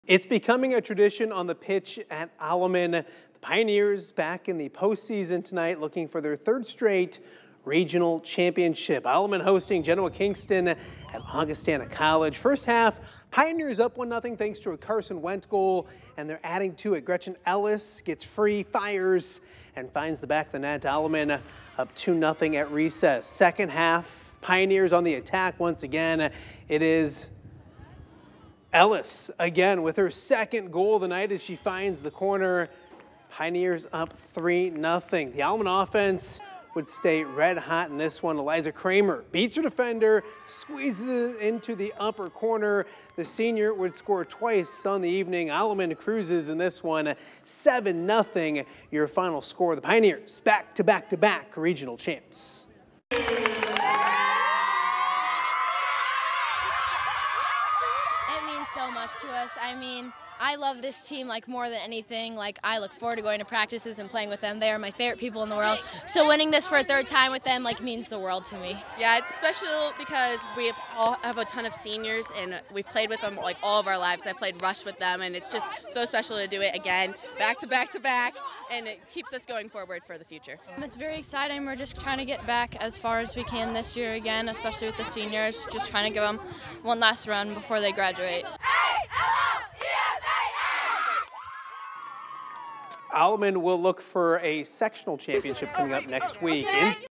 Alleman faces Genoa-Kingston for girls soccer regional title
Alleman cruised to a 7-0 victory, making them back-to-back-to-back regional champs.